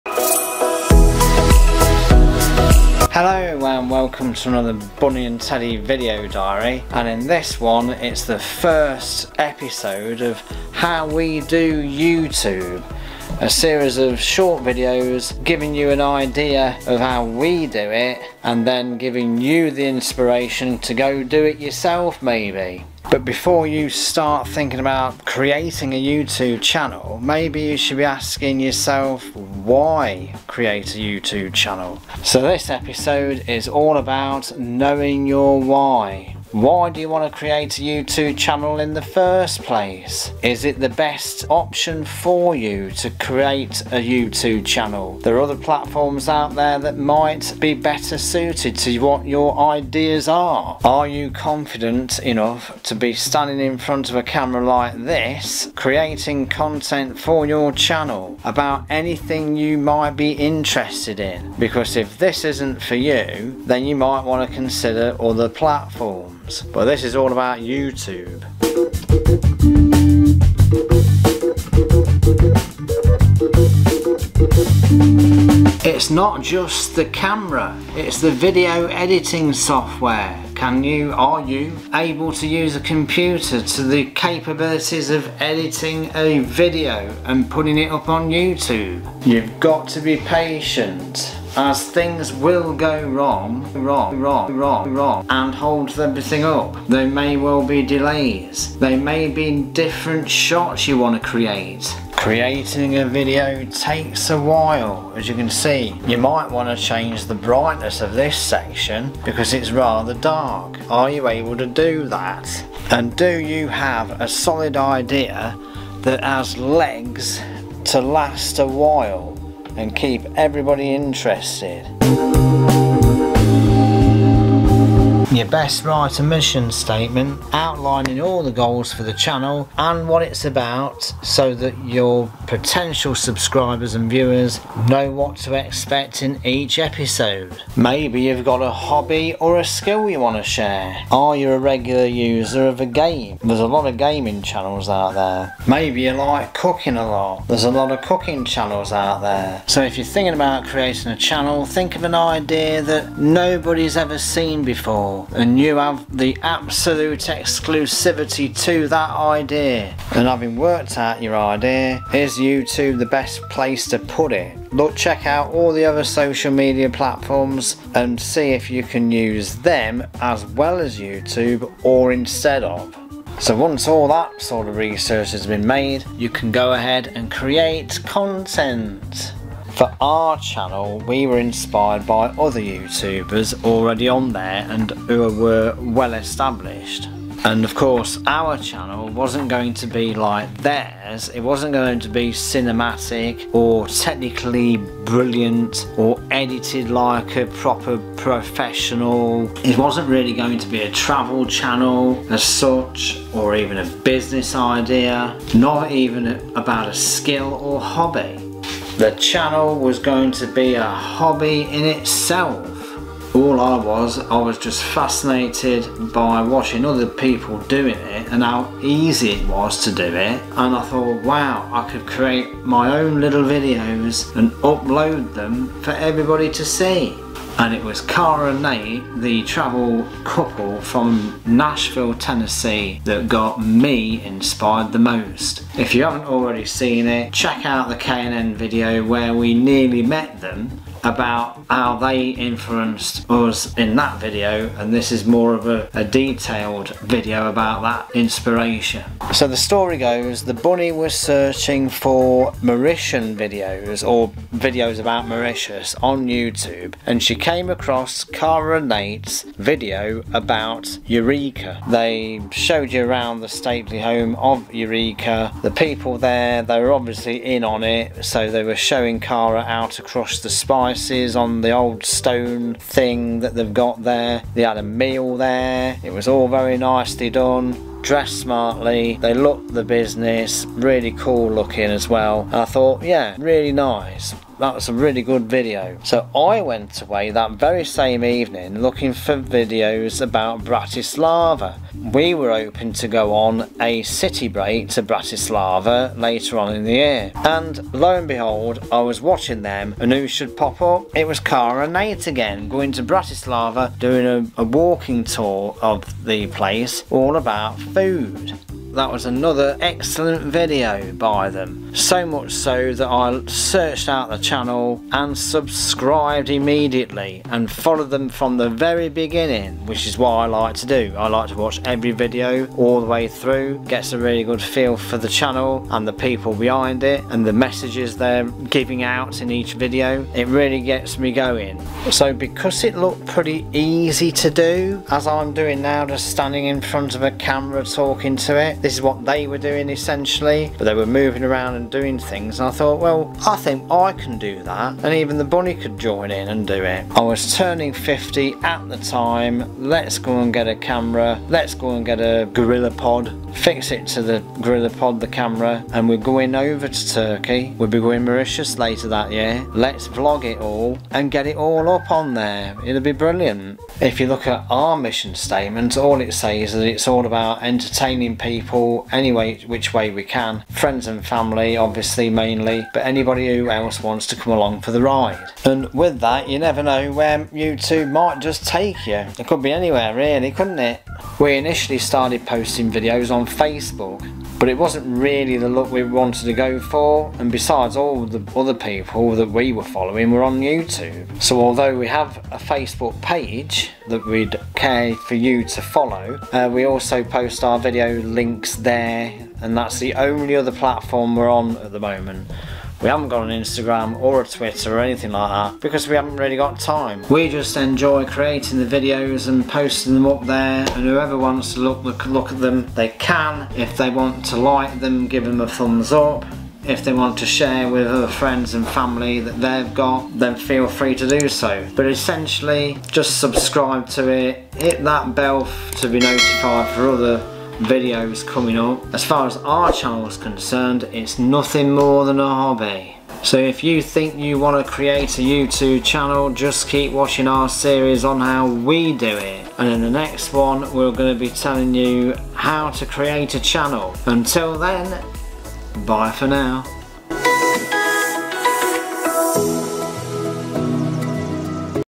HOW WE DO YOUTUBE - KNOW YOUR WHY / GETTING INSPIRED
This is the first episode in a series of short videos about how we do YouTube.
If you thinking of starting your own channel or are just interested to see how we started, then this video may inspire you.
Teddy looks at why you want to start, what skills you may need and how B & T got inspired in the first place.

Check out the end of the video for a couple of links to other videos and a channel link.

This video is by no means an advert for Kara & Nate by the way.  We just love them and their channel.